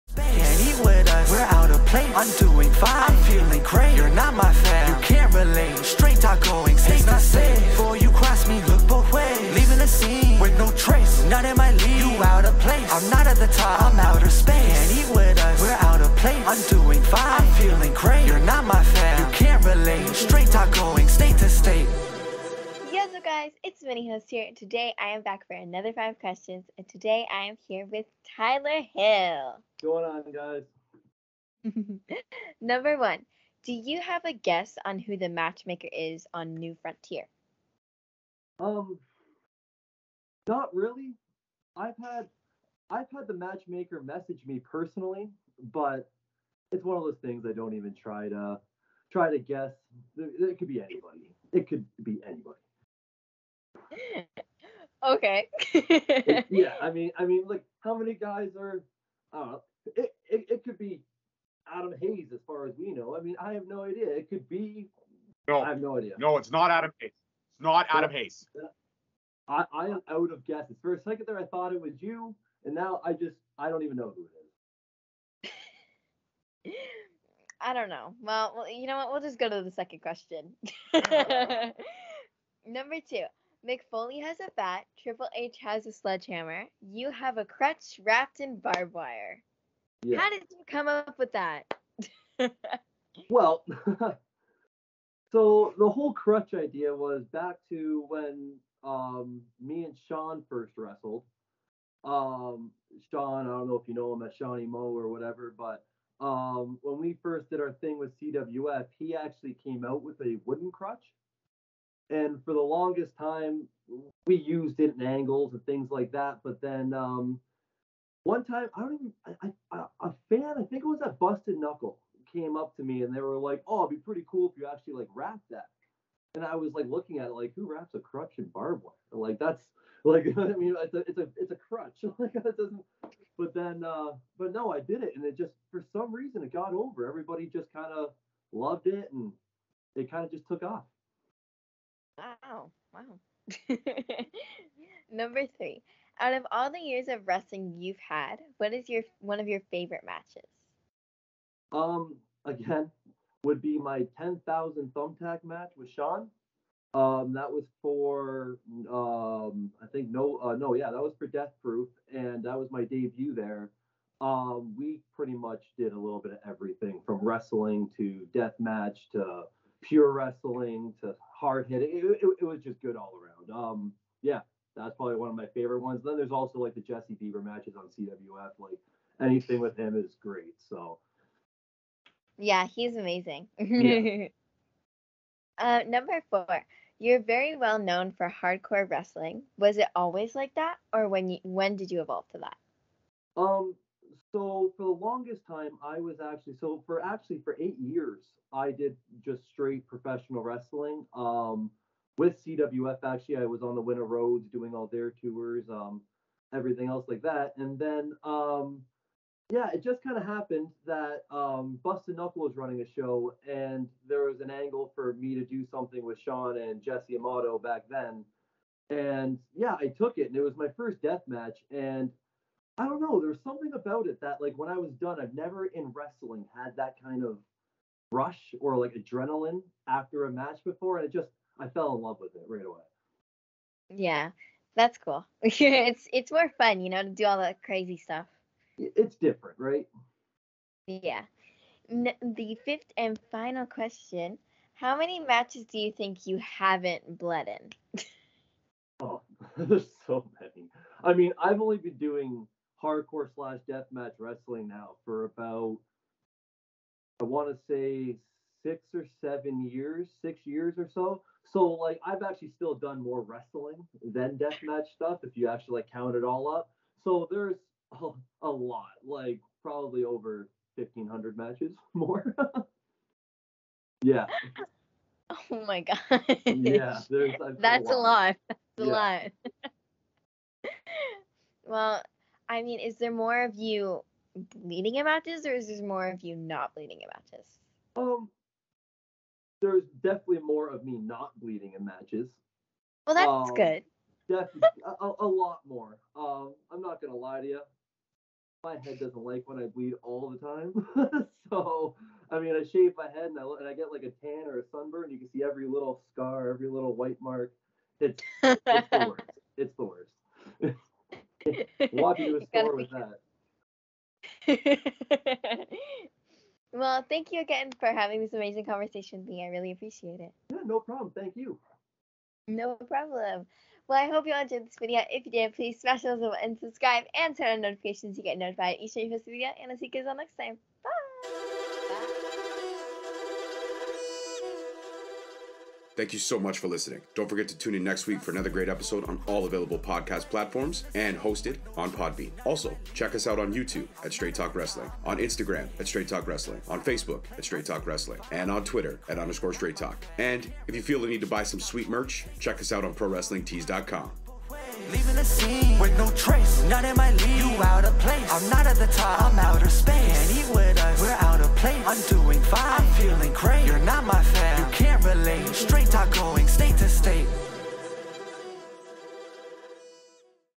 5 QUESTIONS Featuring Tyler Hill
Tyler Hill is in the hot seat as they talk weapons, the matchmaker, and more
Check the LINK TREE BELOW for more content and Merch